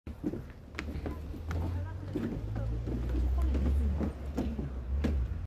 땅과 하늘의 교감
산이 높은 곳은 상대적으로 기압이 낮은 곳이다. 땅위 저지대에 물이 흘러들어 고이는 것처럼 산 높은 곳에는 구름이 저기압 대로 흘러들어 모인다. 이 과정은 매우 역동적이다.